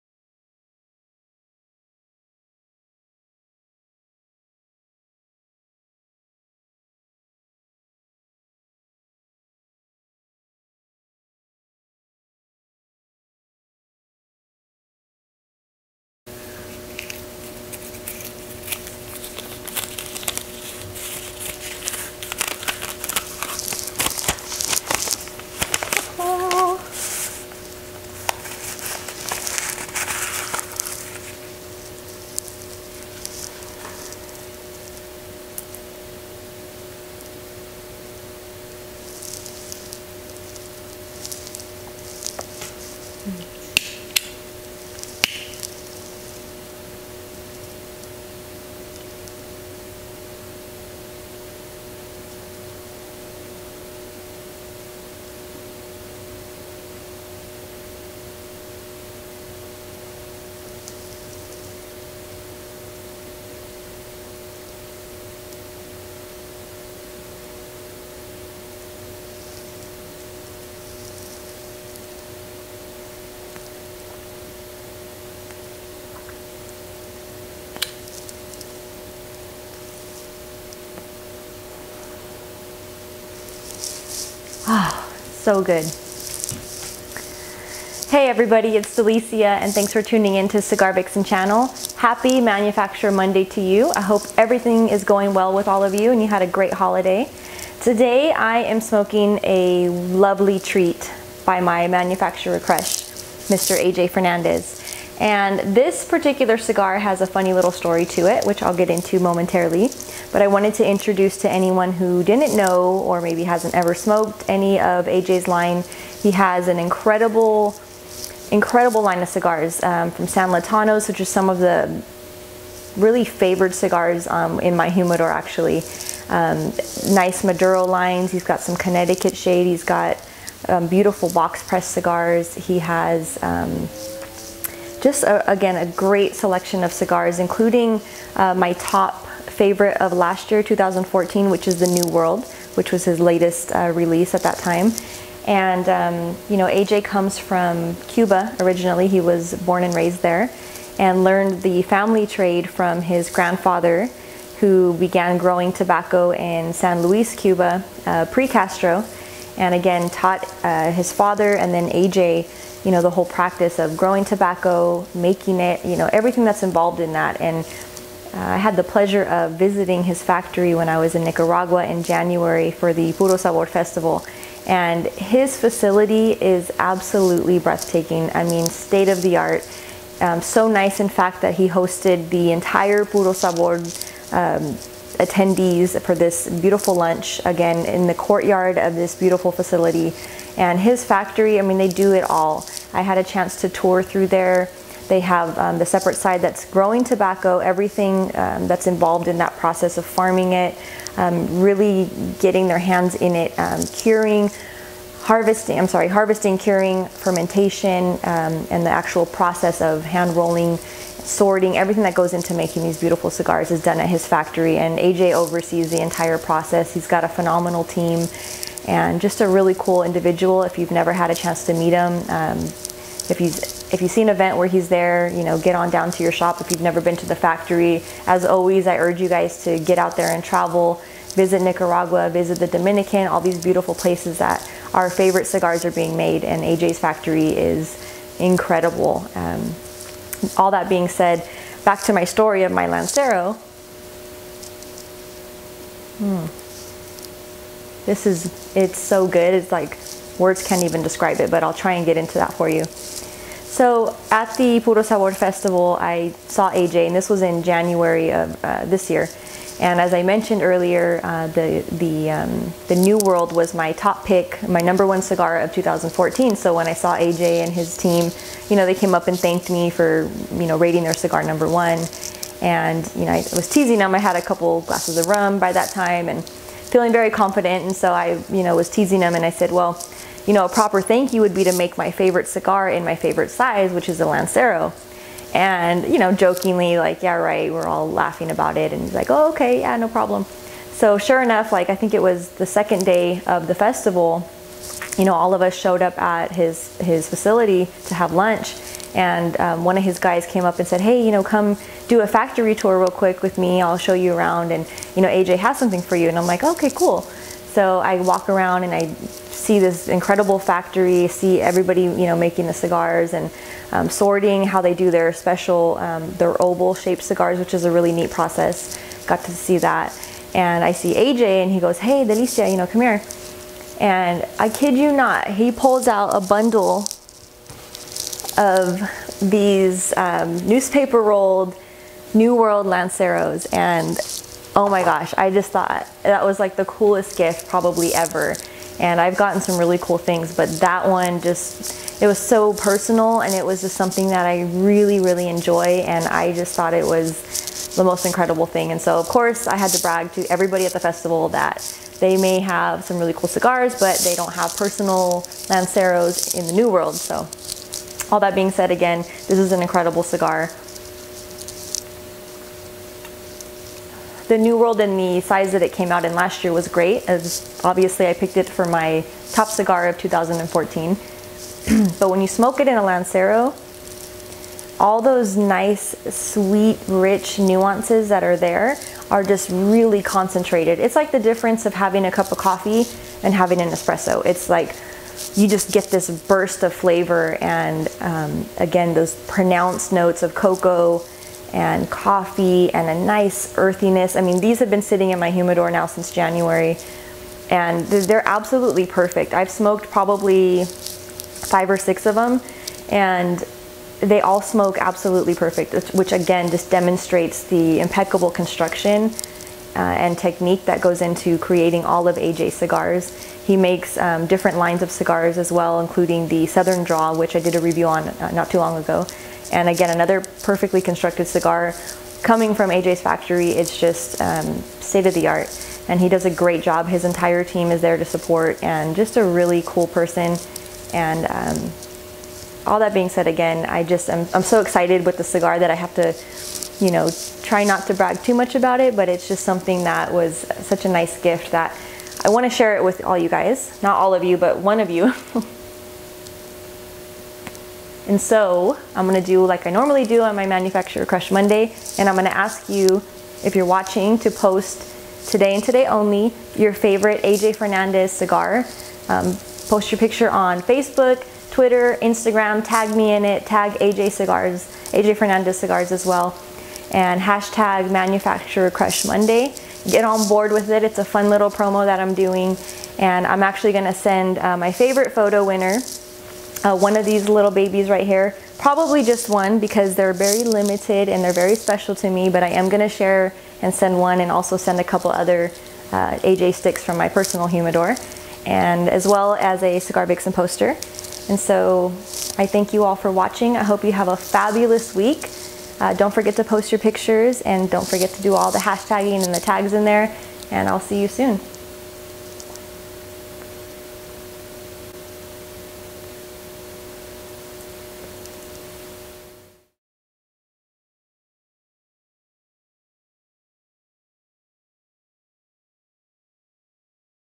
Manufacturer Crush Monday
Post your favorite AJ Fernandez cigar picture to social media (Facebook, Twitter, Instagram) tag me @cigarvixen or Delicia Silva on Facebook, tag @ajfcigars and I will select my favorite picture to win some AJ Fernandez cigars right out of my personal humidor, including a special New World lancero! Don't forget to hashtag